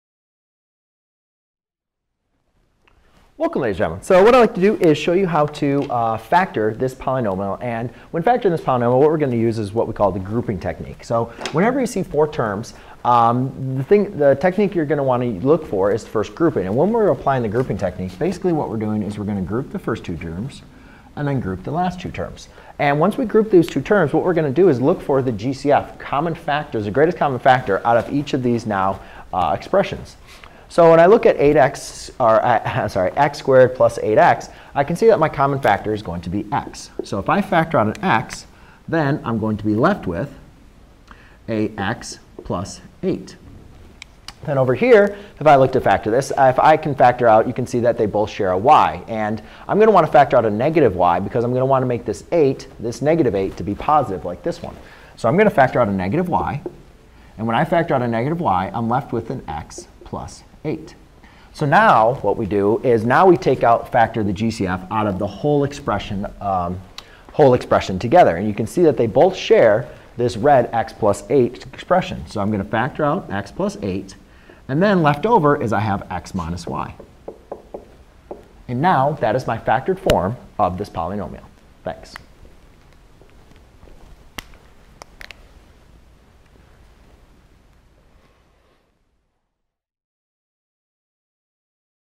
Factoring a polynomial by using the grouping technique
x^2 + 8x - xy - 8y